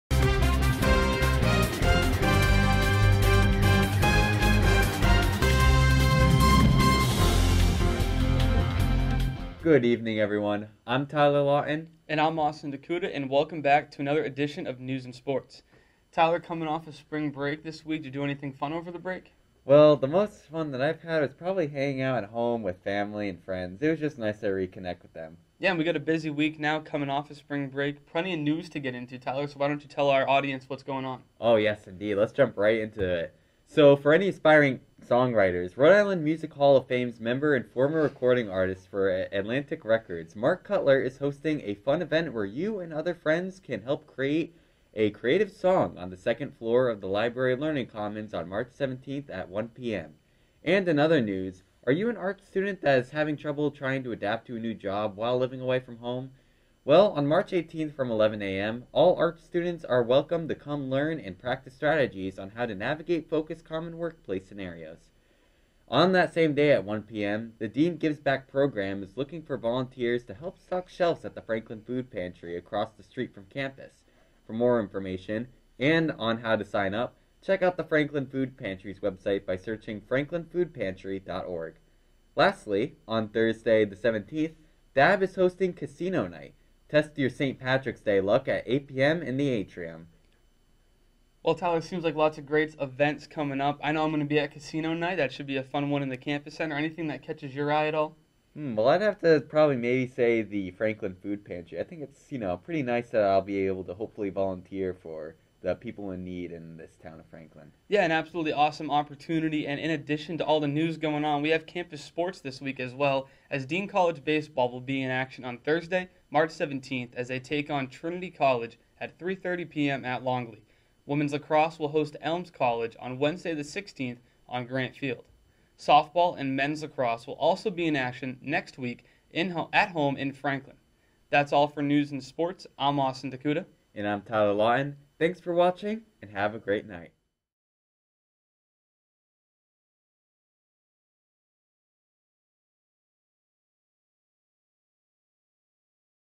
News and Sports: March 21st, 2022
Join Austin DeCouta and Tyler Lawton for the latest in campus News and sports!